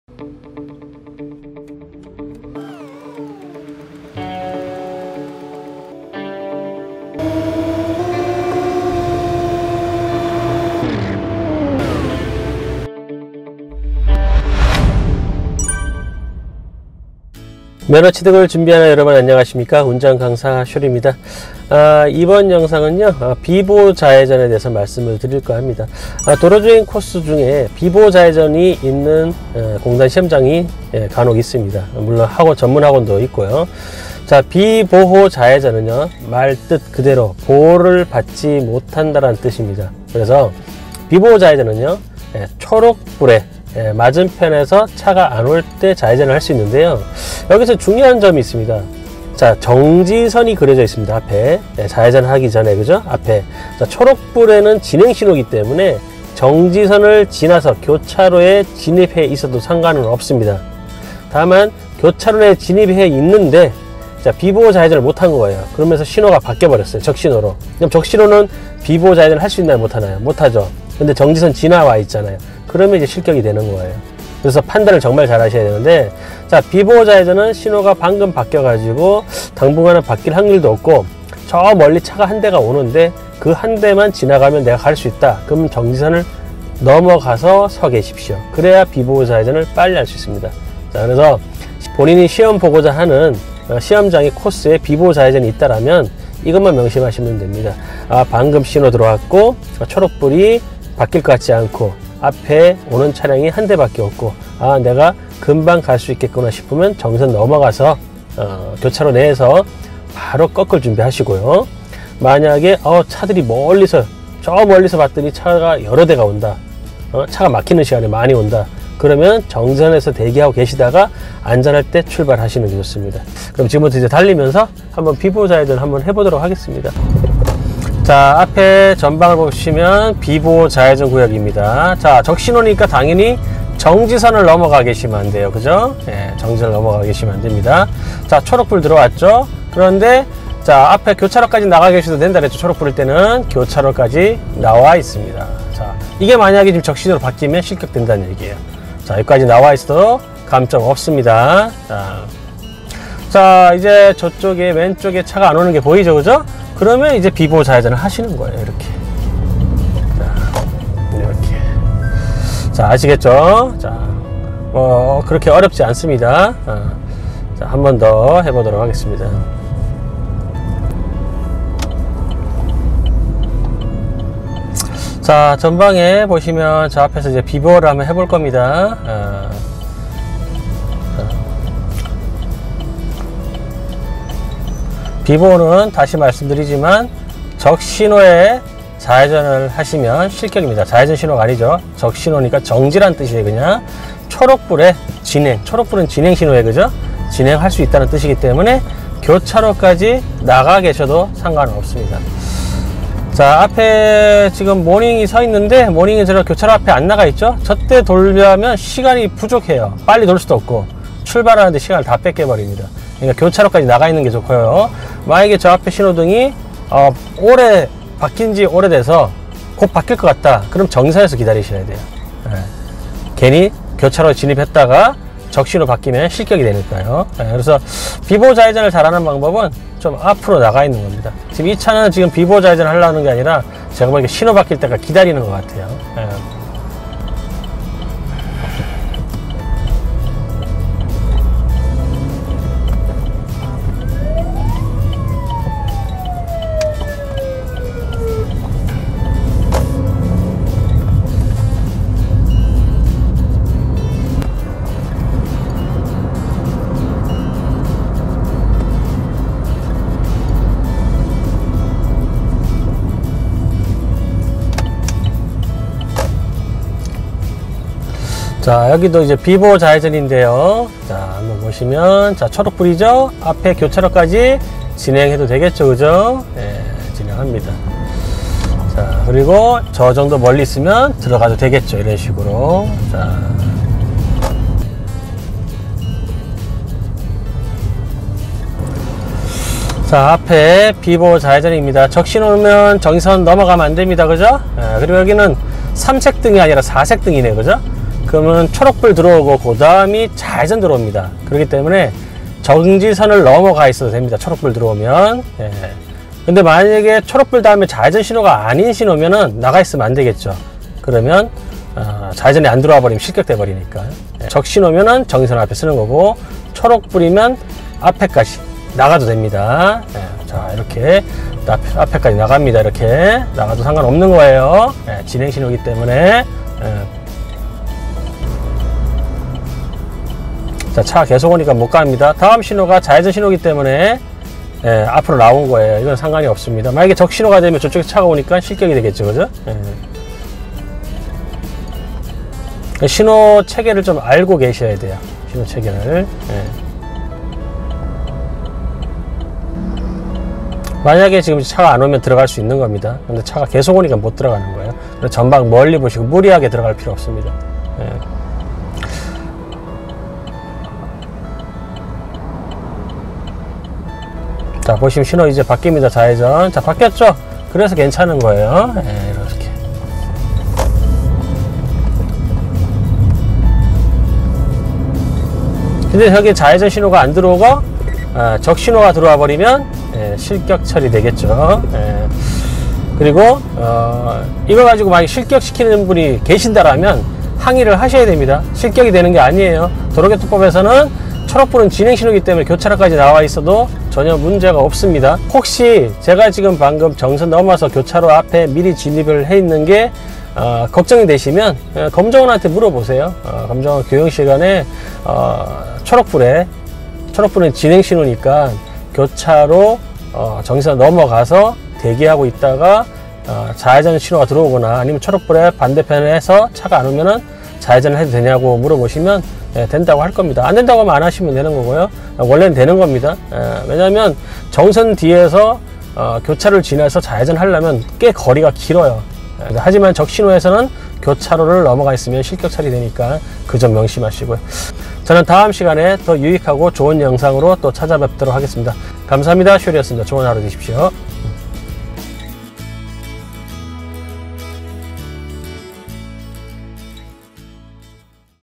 도로주행시험 비보호 좌회전 하는 법
도로주행 코스 중 비보호 좌회전 코스가 있는 분들은 이영상을 참조 하시기
바랍니다. 긍정의 포스가 합격을 함께 응원하겠습니다.

전문학원 검정원 출신의 쇼리쌤의 면허교실 💗~